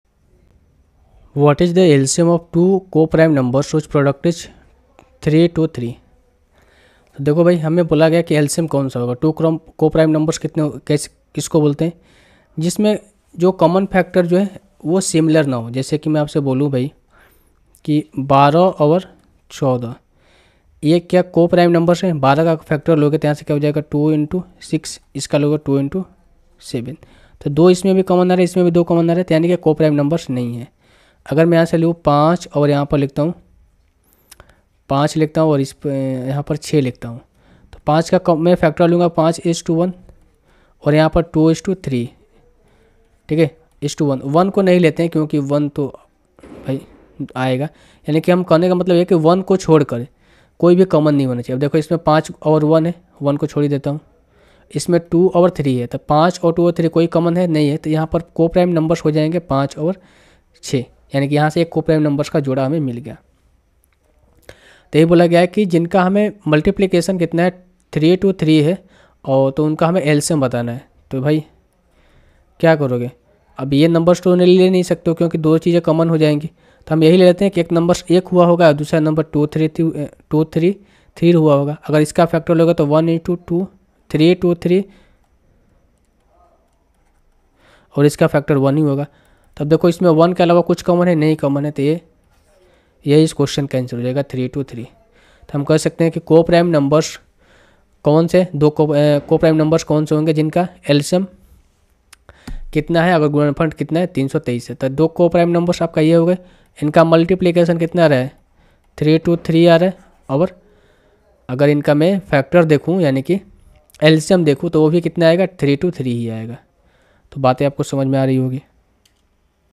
What is the LCM of two co-prime numbers whose product is 323?
दो सह-अभाज्य संख्याओं का लघुत्तम समापवर्त्य (LCM) कितना है,  जिनका गुणनफल 323 है?